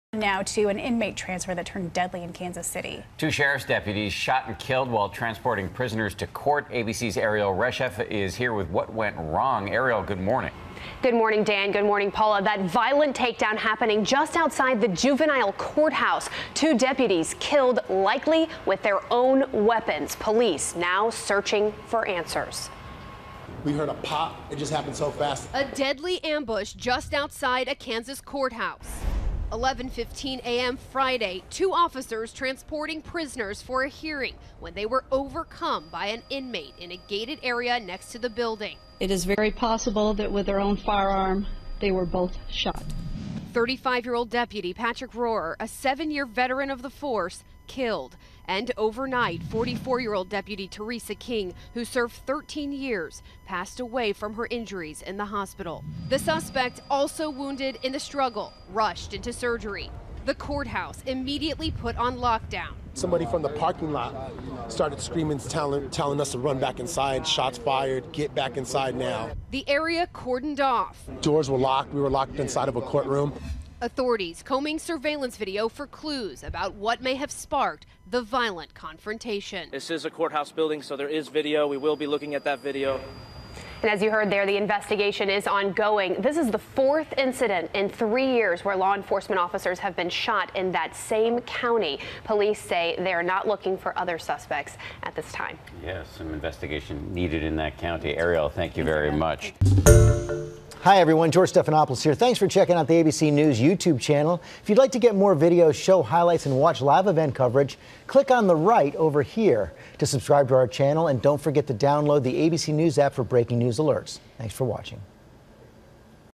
Two Kansas Sheriffs Killed While Transporting Inmates
June 16, 2018 - Deputy Patrick Rohrer and Deputy Theresa King were killed, likely with their own weapons, while transporting inmates to court.